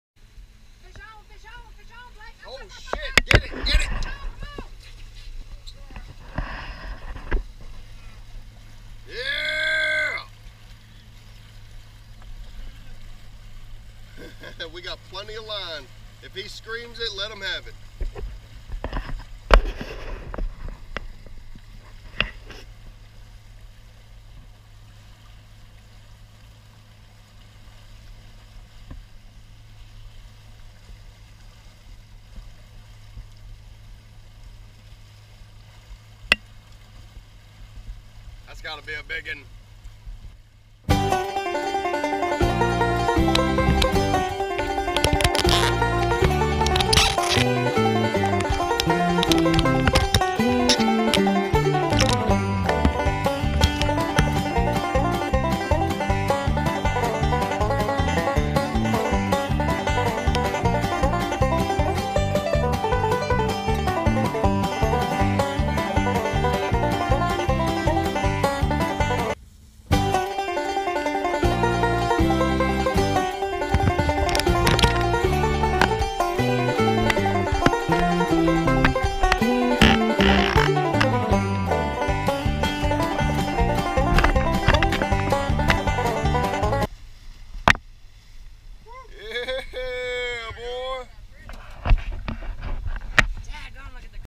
Bolted for Tuna
Trolling October 2017 a mile off the coast of Sneads Ferry NC. Using silver and pink spoons trolling at 5 to 6 knots. Ocean was like glass and the fish were biting! Bolted for Tuna